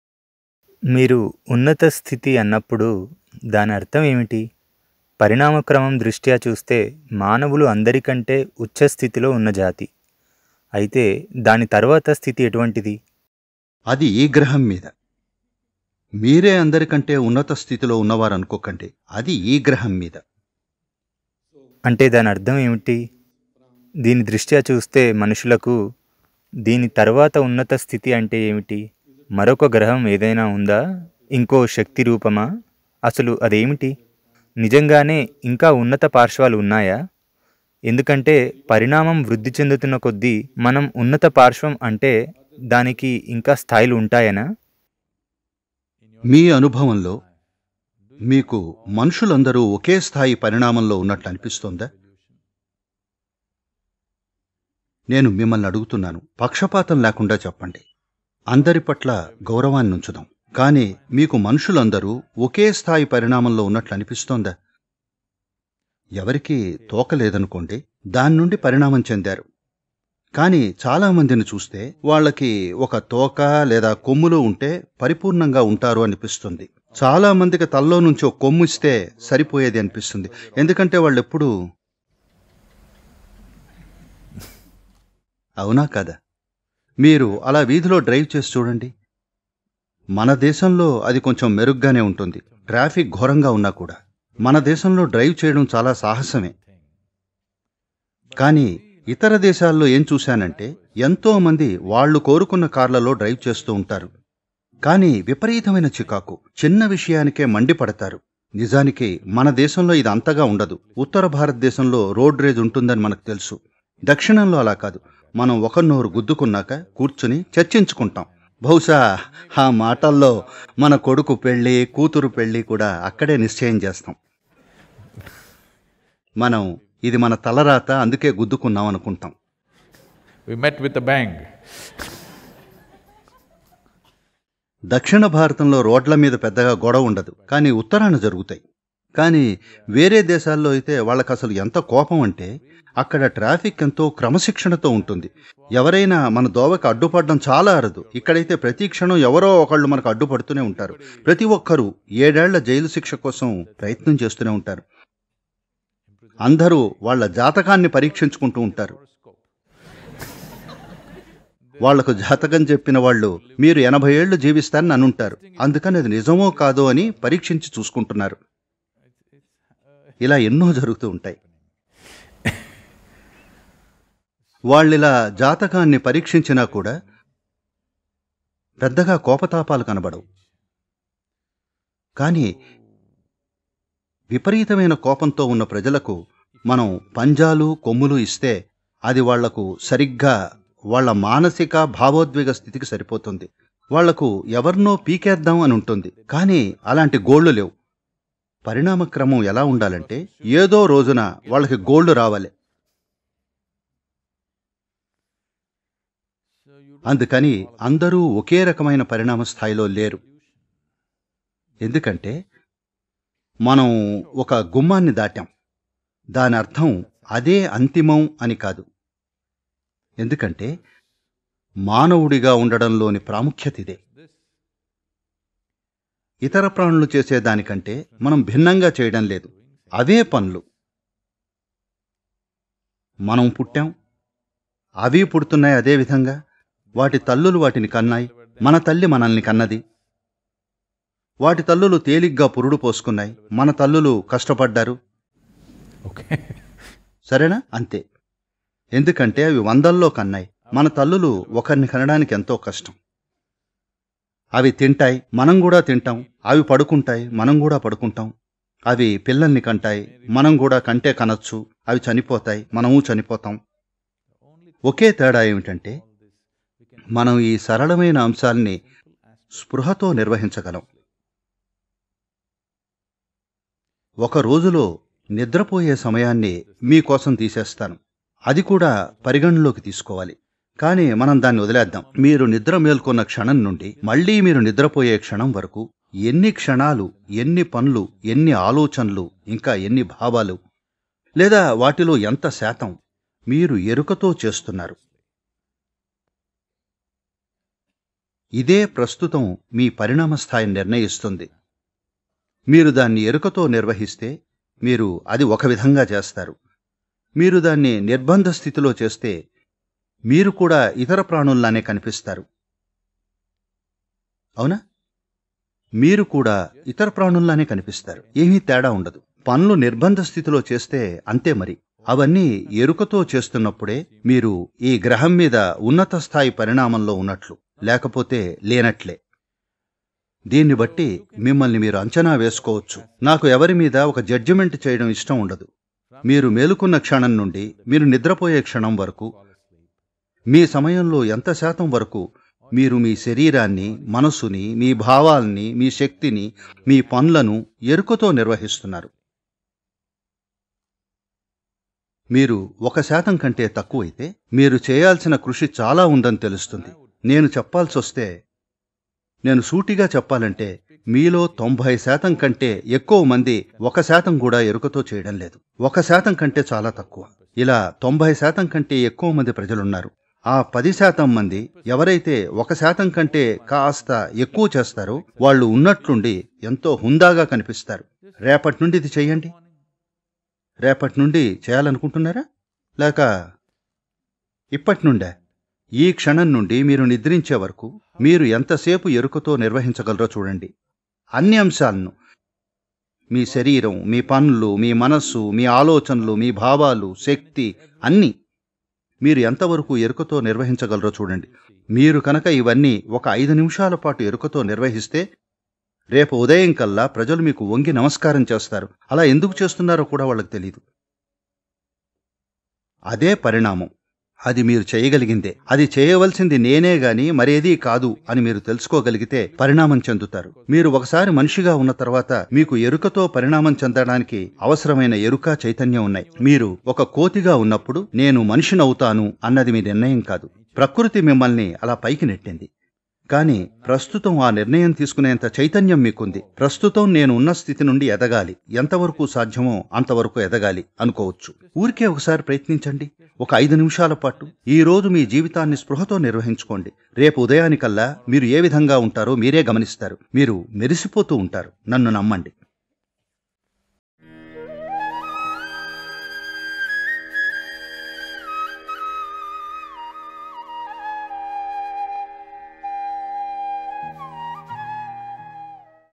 అచేతనం నుంచి చైతన్యం లోనికి! From Unconscious to Concious Evolution | Sadhguru Telugu
"మీరు మేలుకున్న క్షణం నుండి, మీరు నిద్రపోయే క్షణం వరకూ, మీ సమయంలో ఎంత శాతం వరకూ, మీరు మీ శరీరాన్ని, మనస్సుని, మీ భావాలను, మీ శక్తిని, మీ పనులను ఎరుకతో నిర్వహిస్తున్నారు…??? నేను సూటిగా చెప్పాలంటే, మీలో 90% కంటే ఎక్కువ మంది ఒక శాతం కూడా ఎరుకతో చేయడం లేదు. ఒక శాతం కంటే చాలా తక్కువ" అని అంటున్నారు సద్గురు.